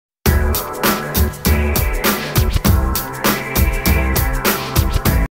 G-ROME BARREL @ SOUTHSHELLY
Gerome getting a nice cover up